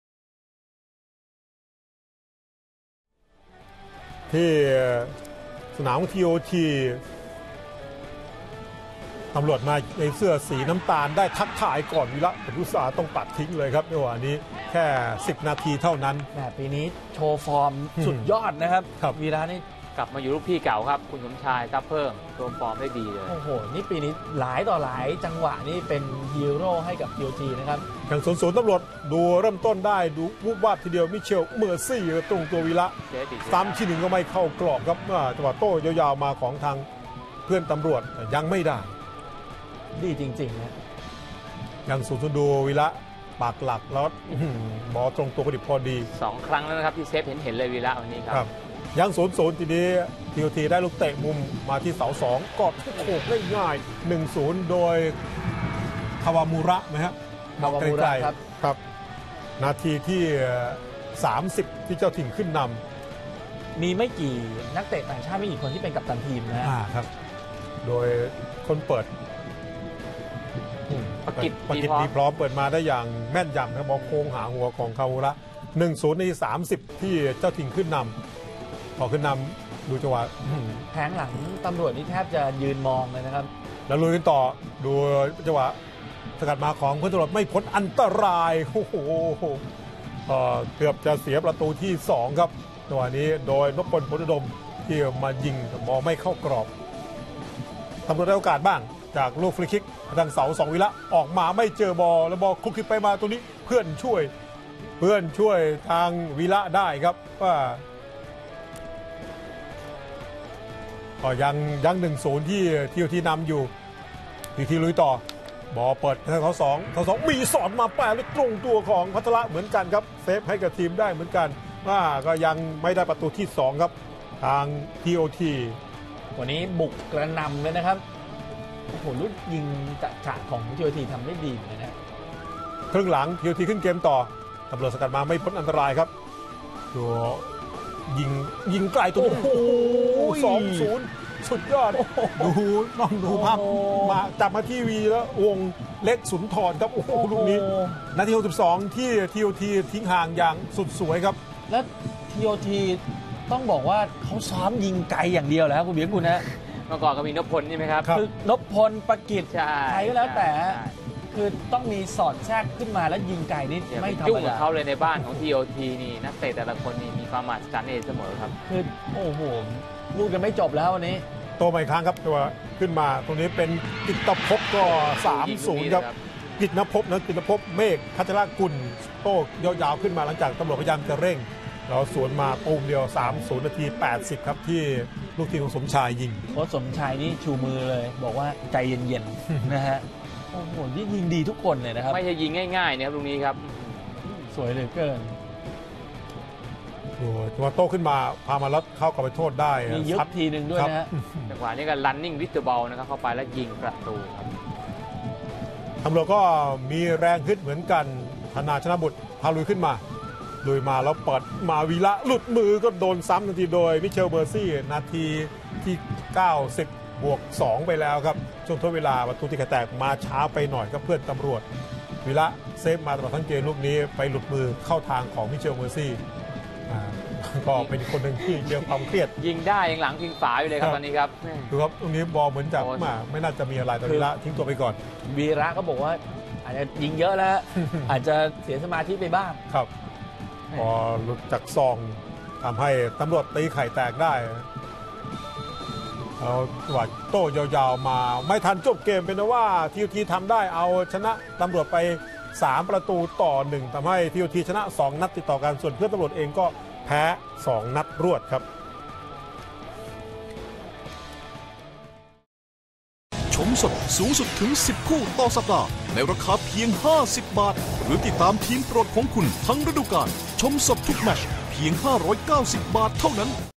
TPL 2014 - TOT 3 - 1 Police United FC (20-04-2014)
TOT 3 - 1 Police United FC
นาทีที่ 33  ทาคาฮิโร่ คาวามูระ   TOT
นาทีที่ 64  วีรวงศ์ เล็กสุนทร
นาทีที่ 82  กฤตณภพ เมฆพัชรกุล

นาทีที่ 90  มิเชล เมอร์ซีย   Police United FC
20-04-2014